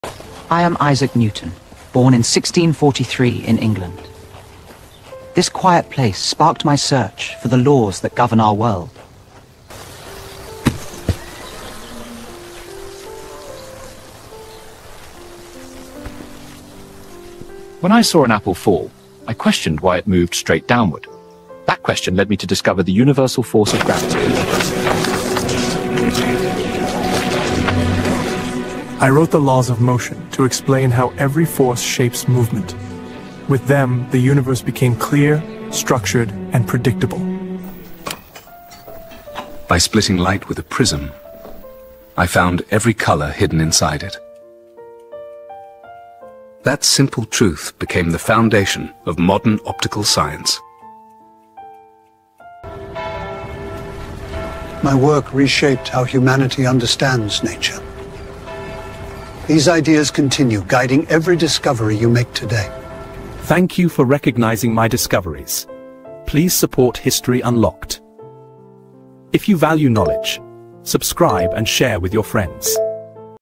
Discoveries That Changed the World - Isaac Newton
Explore the life and groundbreaking discoveries of Isaac Newton, from gravity and the laws of motion to the secrets of light and optics. Witness his journey as a young scholar in England to a pioneer of modern science. This cinematic AI video brings Newton’s story to life with realistic visuals, dynamic camera work, and immersive storytelling.